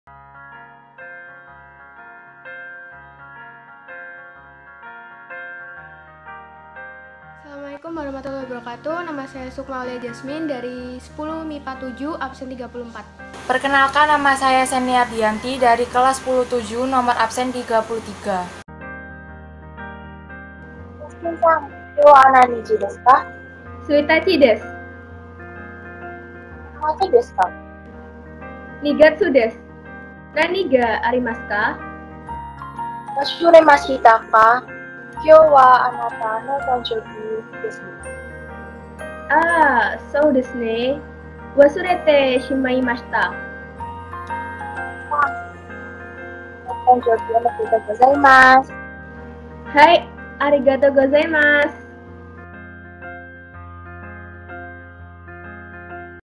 Tugas Bahasa Jepang - Role Play Percakapan mengenai Ulang Tahun dari kelas X-7
Nama kelompok
Sheny Ardianti (33)
Sukma Aulia Jasmine (34)